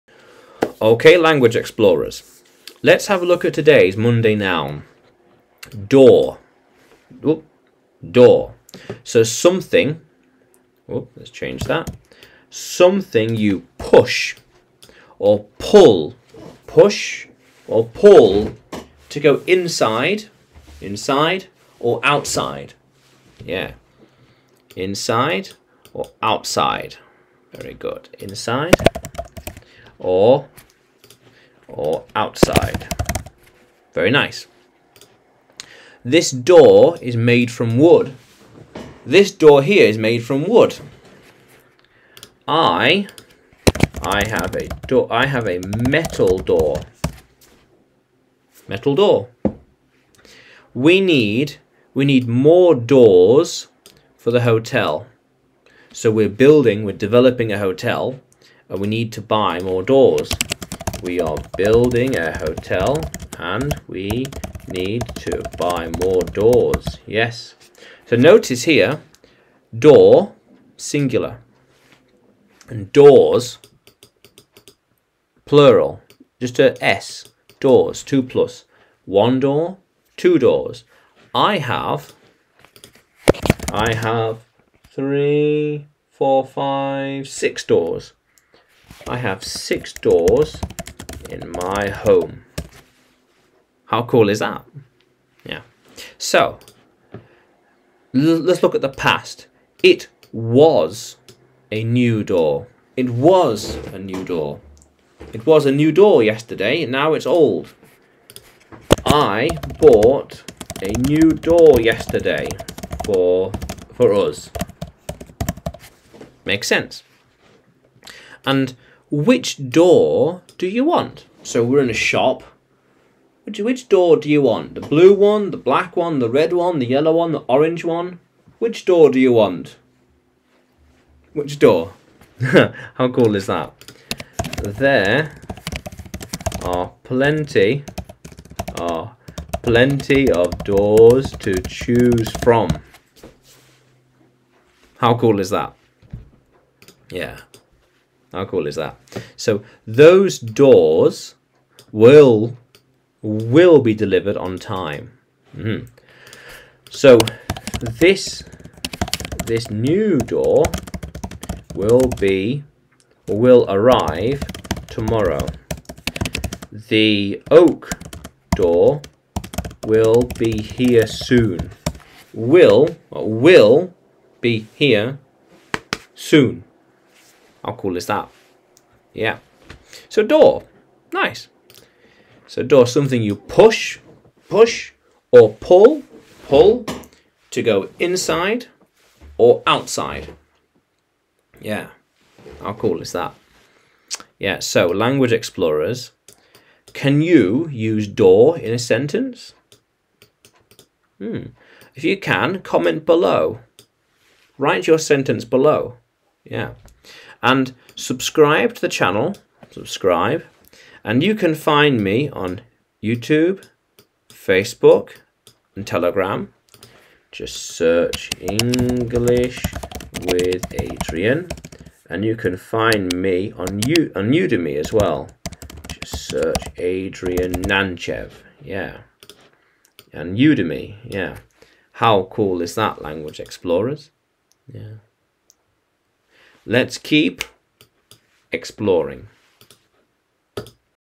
"Door", Nouns in English, Monday Nouns #34
Something you push or
pull to go inside or outside.
This door is made from wood.
We need more doors for the hotel.

Examples, past, present and future:
It was a new door.
Which door do you want?
Those doors will be delivered on time.

Can you write your own sentence in the comments?